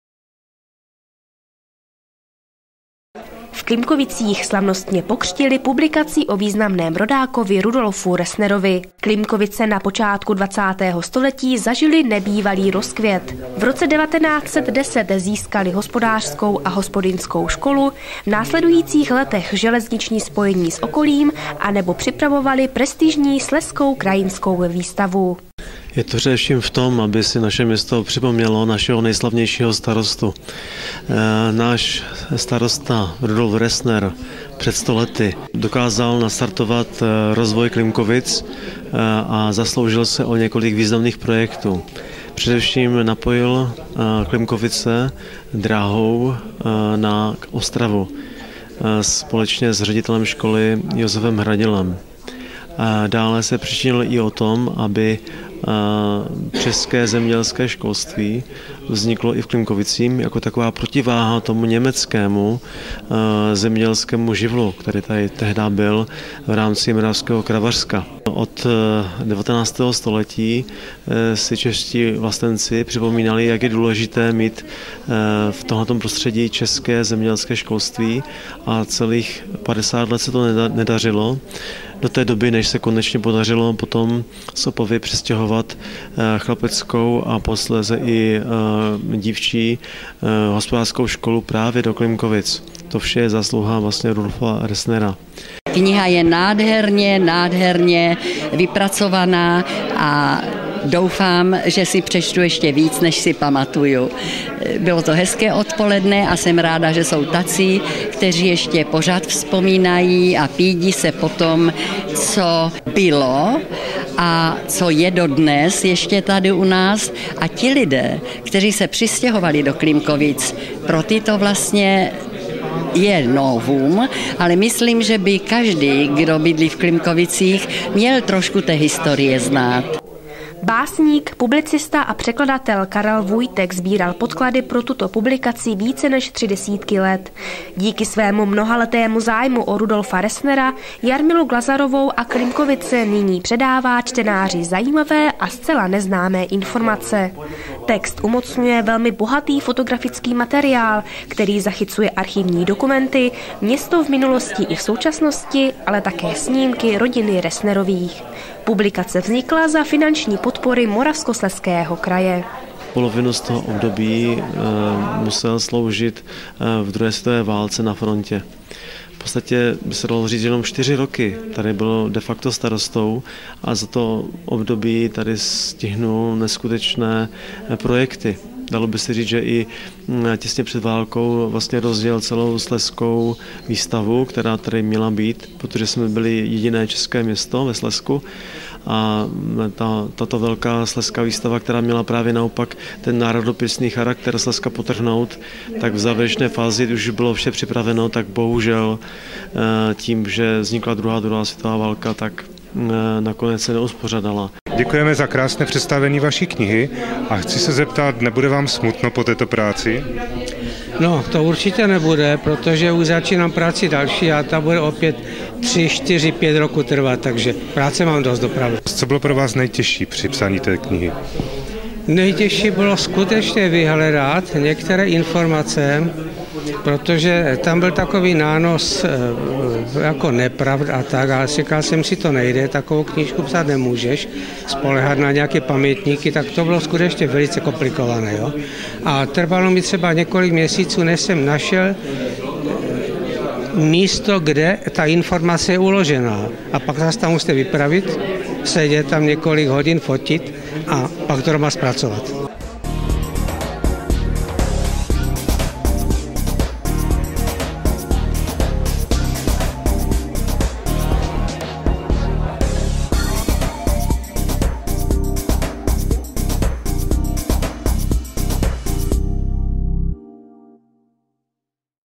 05/2017 - LTV PLUS
Slavnostní křest publikace o Rudolfu Resnerovi.